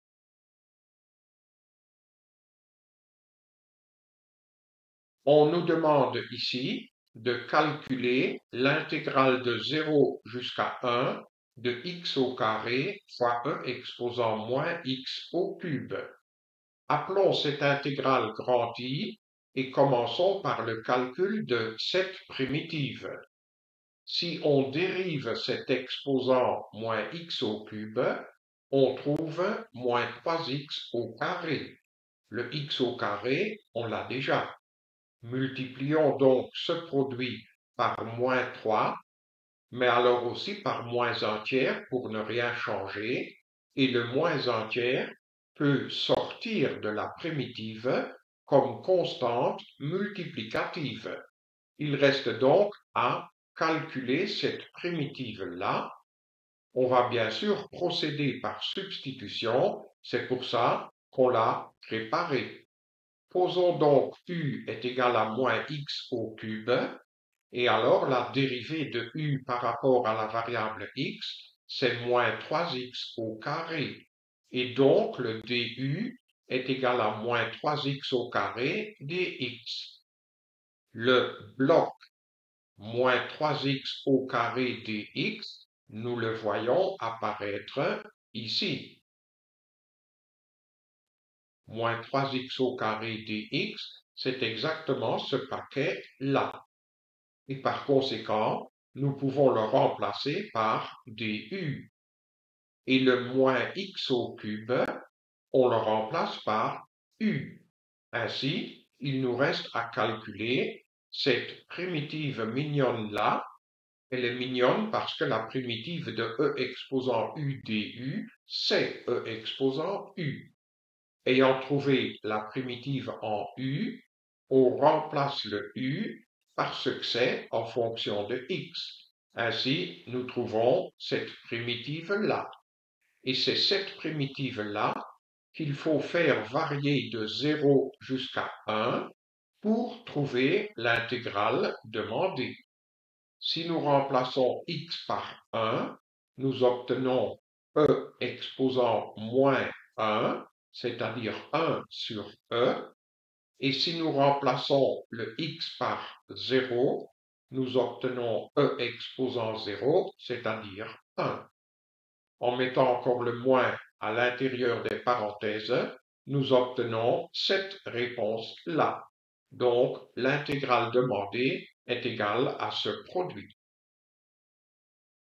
Examen d'admission à l'université de Liège - Analyse 017
Nous répondons à une sous-question d'analyse concernant une intégrale, qui a été posée en 2023 lors de l'examen d'admission aux études d'ingénieur civil.